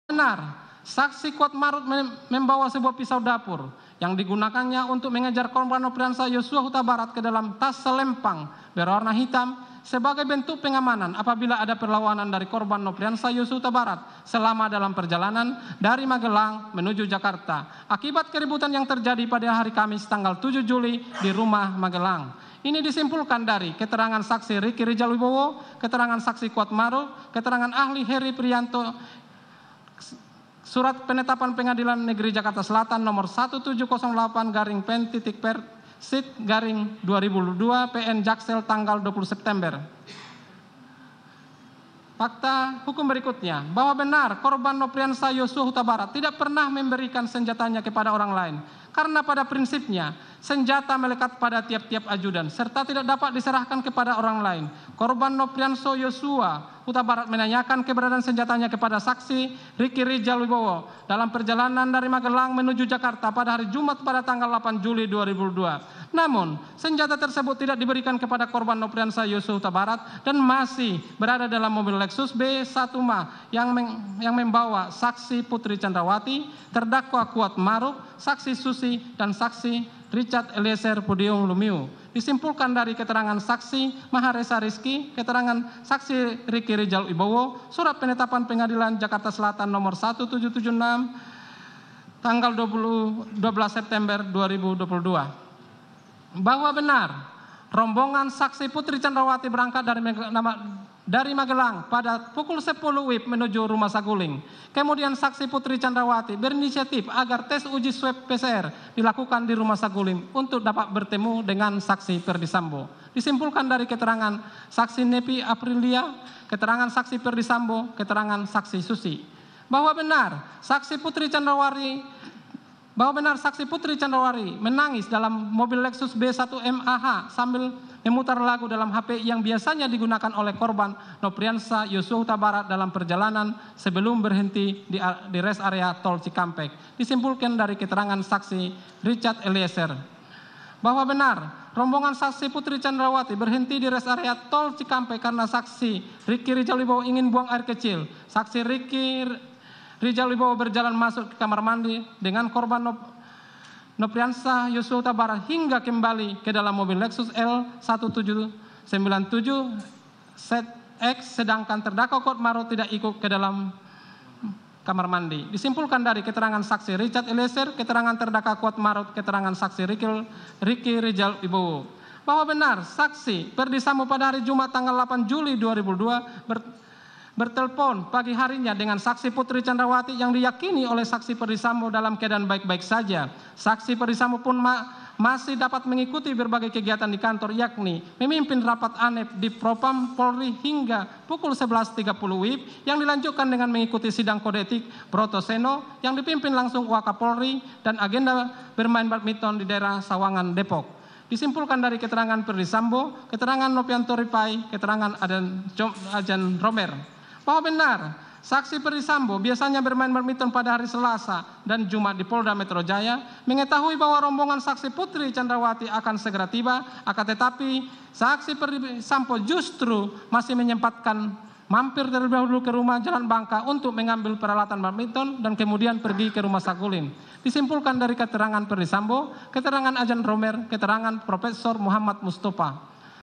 Jaksa Beberkan Prilaku Putri Candrawathi di Perjalanan Magelang menuju Jakarta
JAKARTA, KOMPAS.TV - Dalam perjalanan menuju Jakarta dari Magelang, Putri Candrawathi menangis sambil memutar lagu yang biasa diputar oleh Yosua Hutabarat.

Selain itu, Kuat Ma'ruf masih membawa dua buah pisau untuk berjaga-jaga jika Brigadir J melawan.

Jaksa Penuntut Umum membacakan tuntutan kepada Kuat Ma'ruf dan Ricky Rizal pada sidang Senin hari ini (16/1).

Dalam keterangan sidang sebelumnya, Ricky Rizal dan Kuat Maruf kompak merasa tak bersalah atas kematian Yosua.